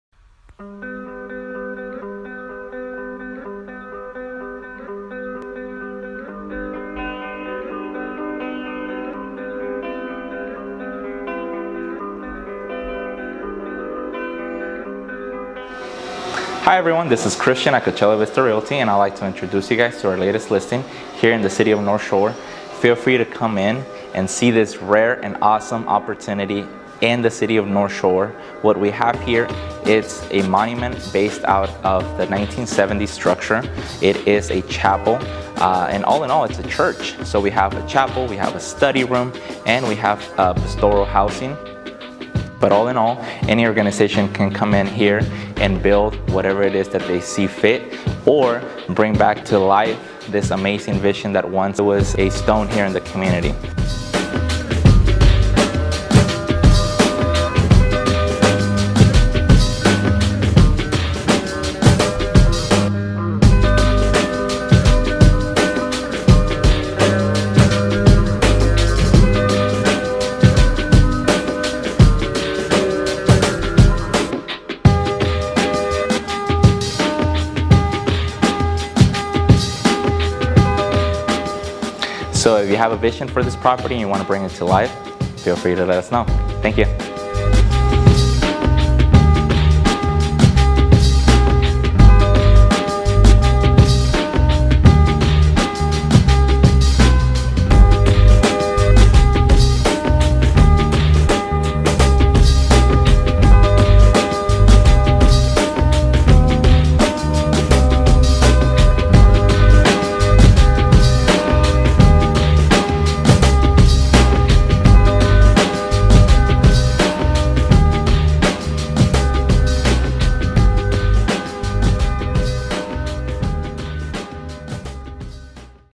Coachella Valley Real Estate Video Services
At 87 Innovations, we specialize in real estate photography, videography and Matterport 3D Tours. Our staff utilizes the newest 4K video technology, cameras, drones, and editing software to provides you with the highest quality media to showcase your listings. Our packages include:

- Interior & exterior photos
- Drone photos
- Branded & Non-Branded videos
- Drone Video
- Animated markers on points of interest